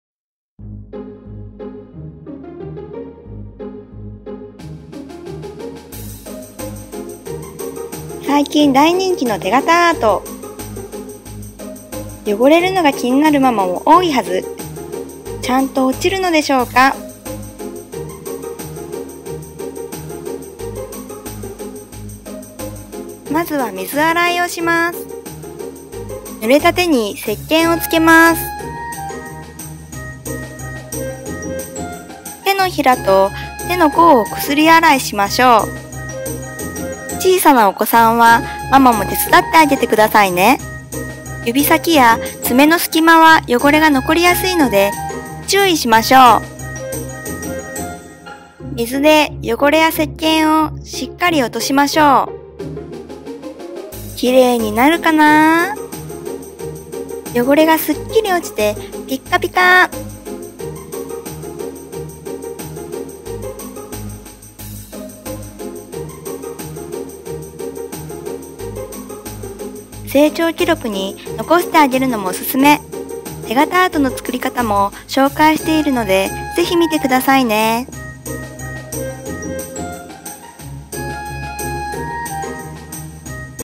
子どもの手についたインク汚れ☆手洗いでキレイに落ちるの？
"大人気の手形アート！作ってみたいママも多いはず。けれど、手形や足形を取った後のインク汚れが落ちるのか心配ですよね…。今回は手洗いの方法でキレイに落ちるか実際にしてもらいました。正しい方法を覚えて可愛い赤ちゃんの手形を作ってあげましょう♪手形アートの作り方の動画もあるので、是非ご覧ください。

この動画の関連情報「おすすめメモリアルグッズ」